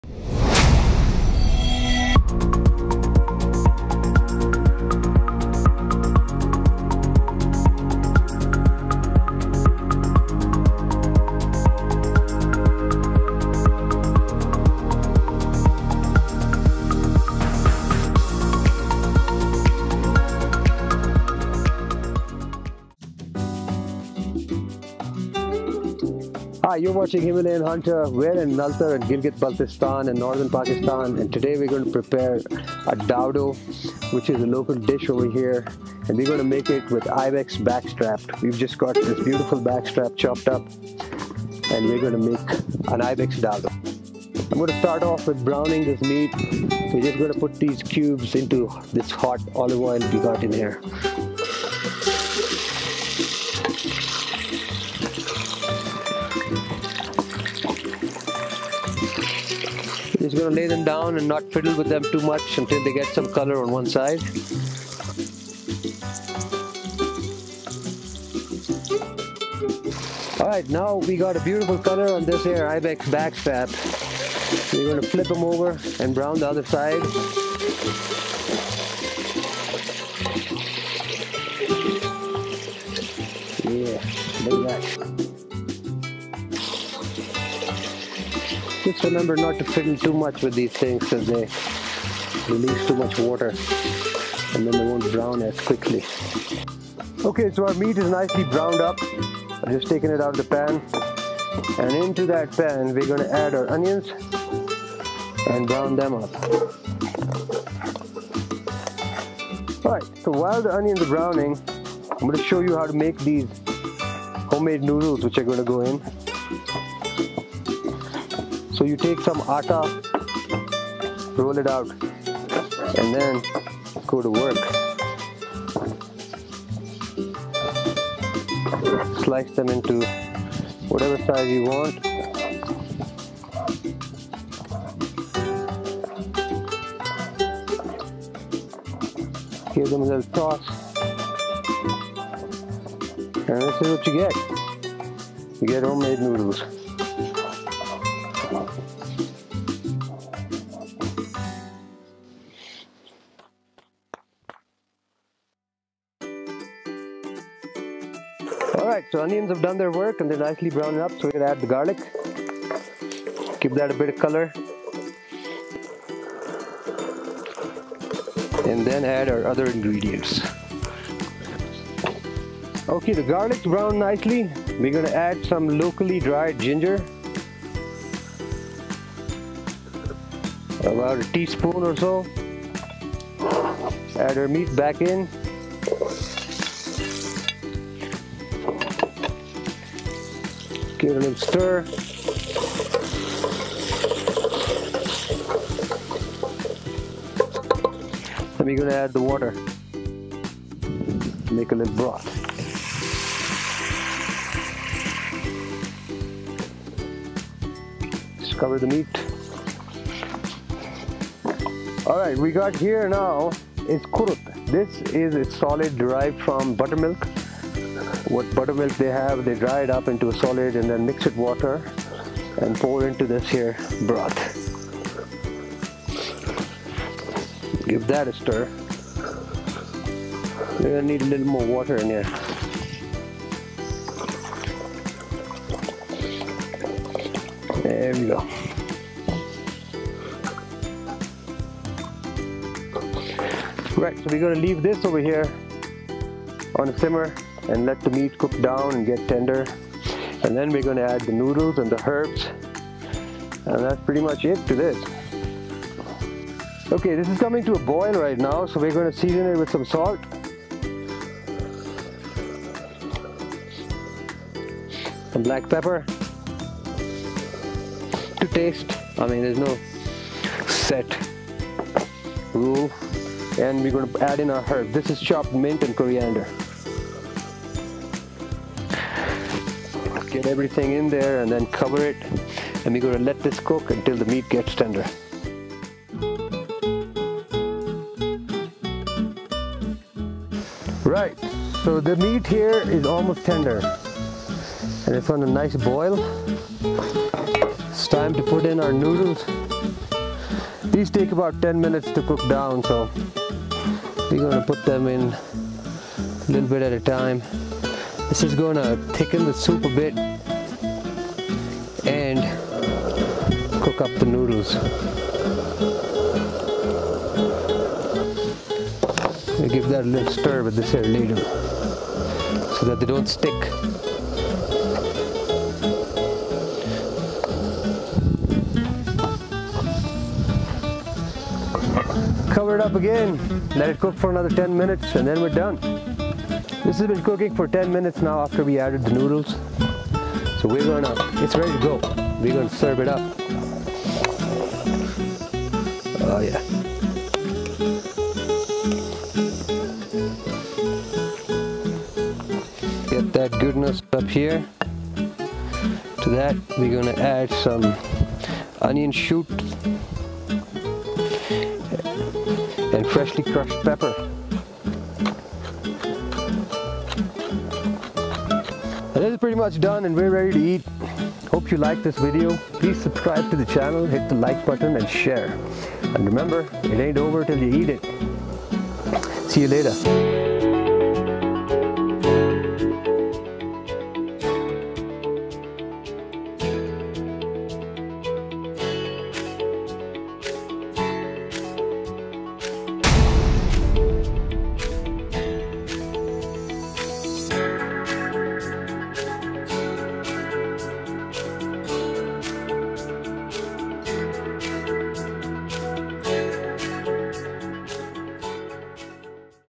The Himalayan Hunter: Cooking Himalayan Ibex Soup 'Dowdo'
The Himalayan Hunter is cooking Himalayan Ibex soup 'Dowdo', after a hunt.